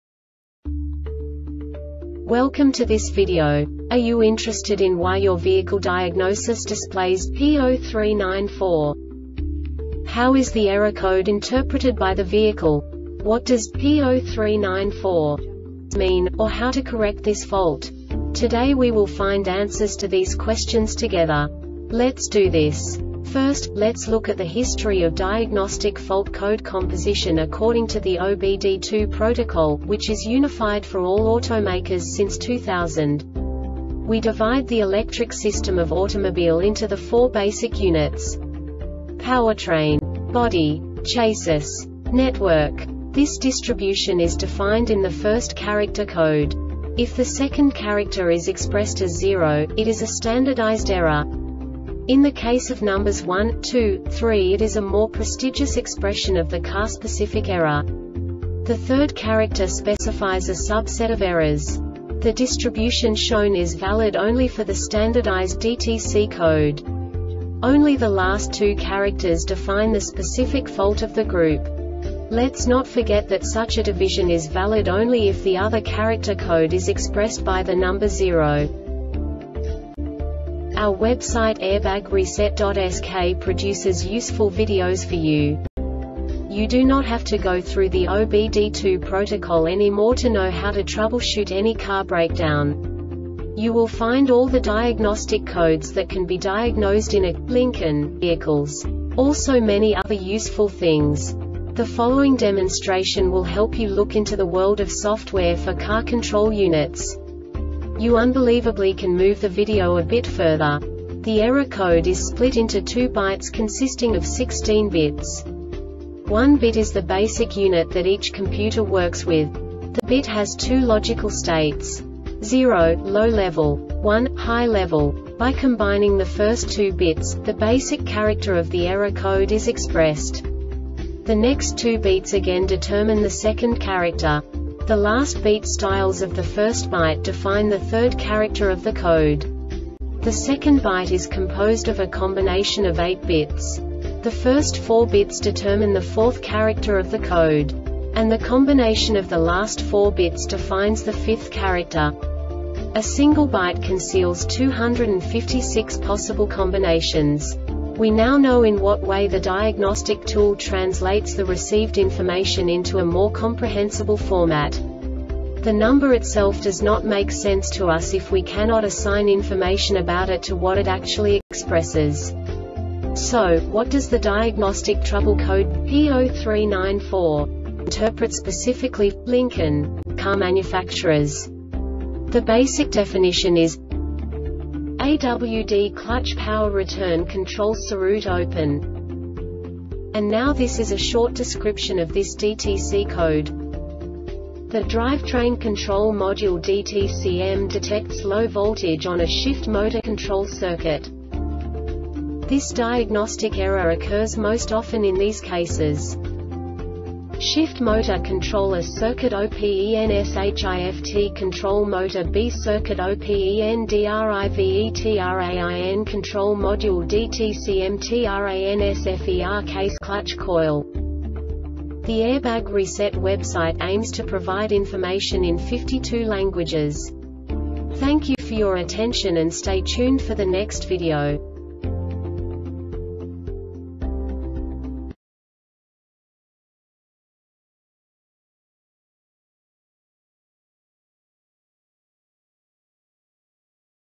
DTC Lincoln P0394 Short Explanation
Contents:
0:21 Basic DTC analysis according to OBD2 protocol standard.
1:48 Insight into programming
2:58 A diagnostically understandable description of the Diagnostic Trouble Code
3:05 Basic error definition

Make:
Lincoln
Code:
P0394
Definition:
AWD-Clutch-Power-Return-Control-Ciruit Open
Description:
The Drivetrain Control Module (DTCM) detects low voltage on a Shift Motor Control circuit.
Cause:
SHIFT MOTOR CONTROL A CIRCUIT OPENSHIFT CONTROL MOTOR B CIRCUIT OPENDRIVETRAIN CONTROL MODULE (DTCM)TRANSFER CASE CLUTCH COIL